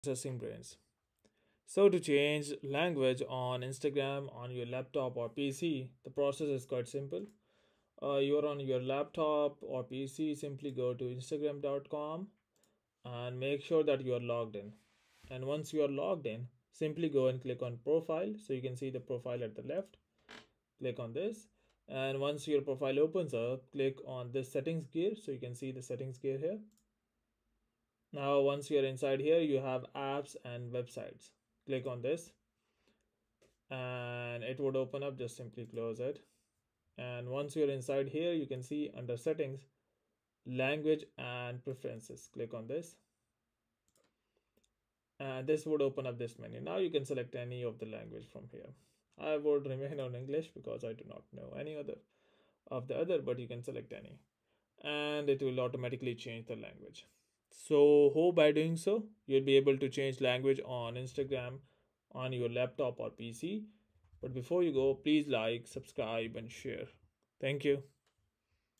How to Change Language on Instagram on Laptop/PC (Quick & Easy)
Learn How to Change Language on Instagram on Laptop/PC, simply follow this video.

0:00 Intro
0:05 How to Change Language on Instagram on Laptop/PC